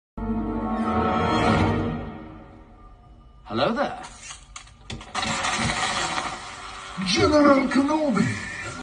Hello there! But in real life
Hello there General Kenobi, is reacted in real life by disney
not my vid